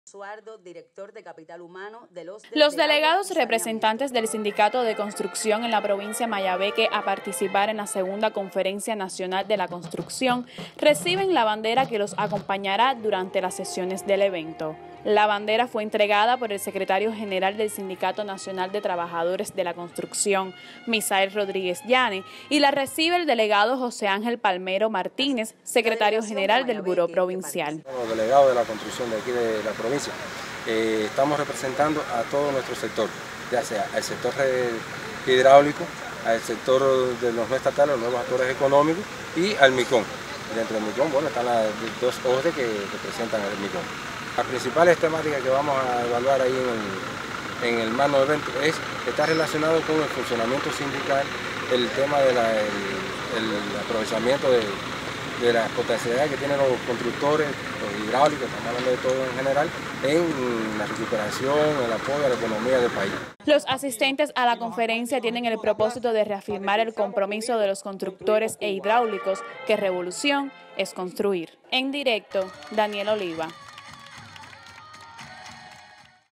Abanderan a Delegado del Sindicato de la Construcción en Mayabeque.
Abanderan a delegados del sindicato de la construcción, los cuales representaran a la provincia Mayabeque en la Segunda Conferencia Nacional de la construcción.